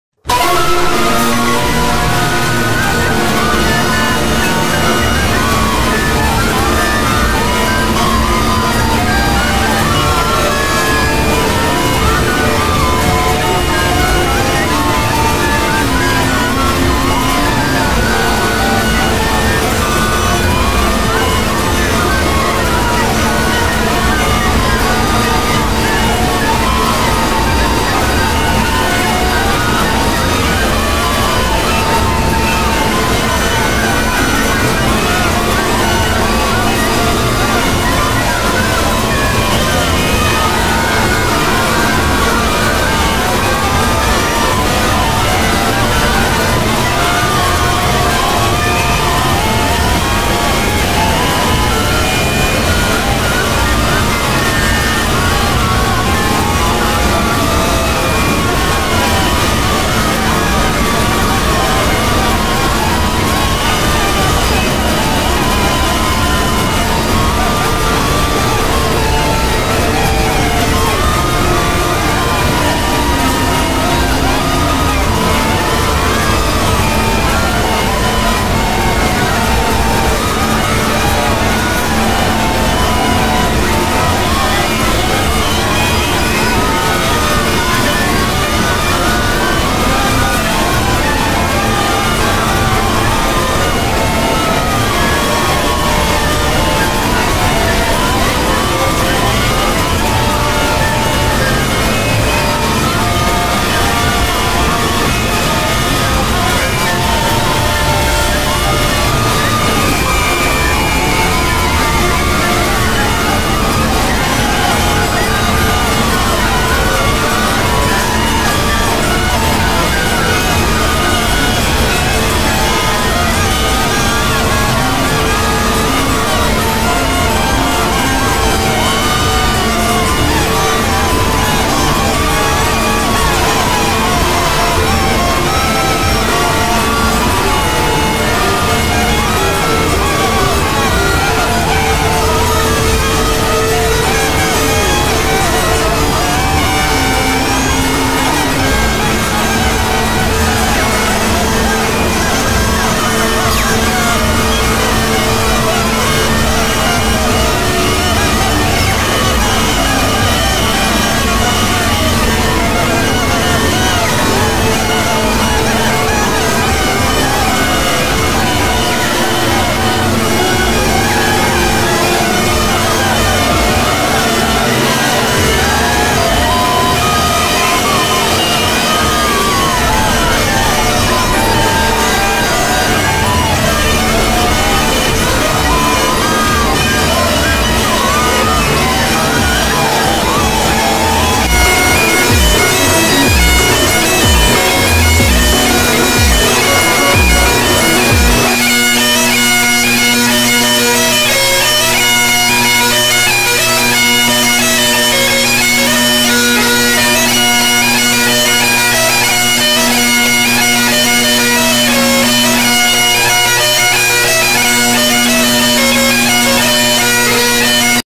Earbleed Tool To Use
Most of these are just earrape versions of video game music or other tunes, but the flute tunes are made by shittyflute, go sub to him he makes great content